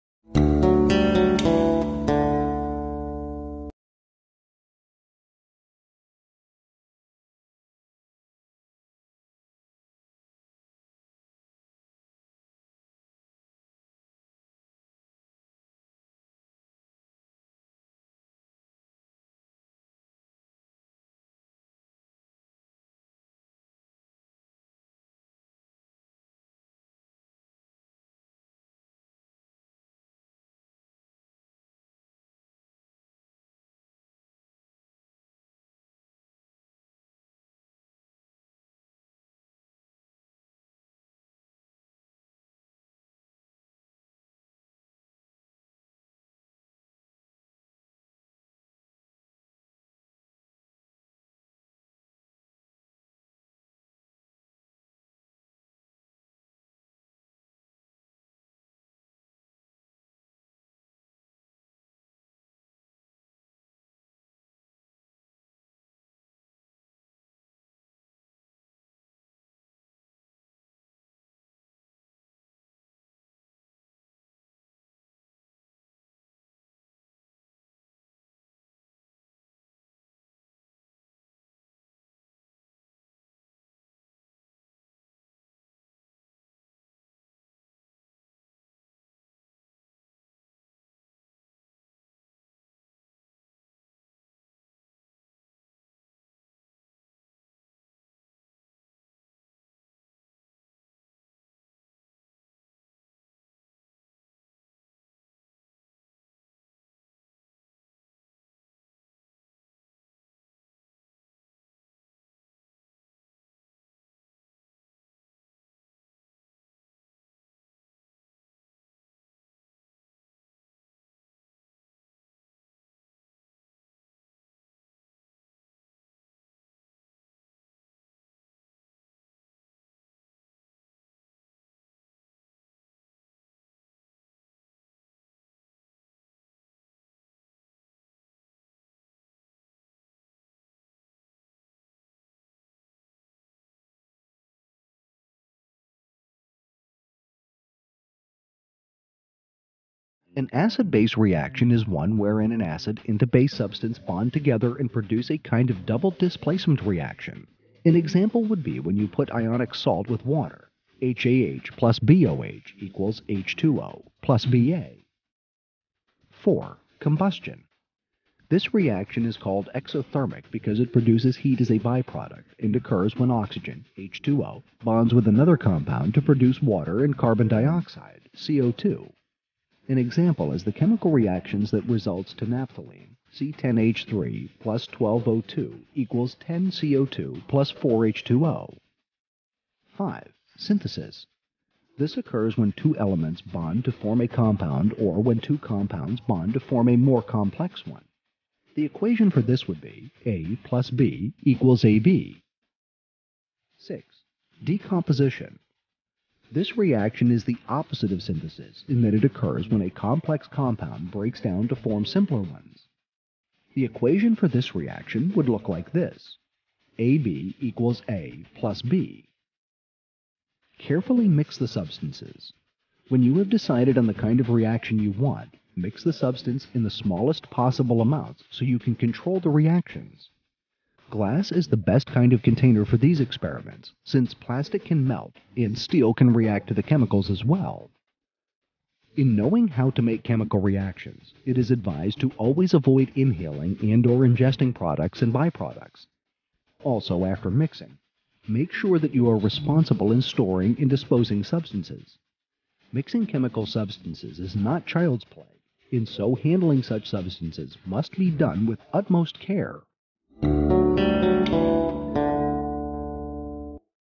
How To Make Chemical Reactions
Tutorial on how to make chemical reactions. Knowing how to make chemical reactions entails knowing which substances will react to each other and how. It also requires knowledge of basic chemistry. It must be said that mixing chemical substances can be dangerous business since some products (whether accidental or intentional) can be toxic. So while it can be fun to see smoke and colors change as a result of chemical reactions, experimenting with chemical substances is to be done with all necessary precautions such as using the right apparatus and containers for the chemicals as well never handling them with your bare hands. Waysandhow.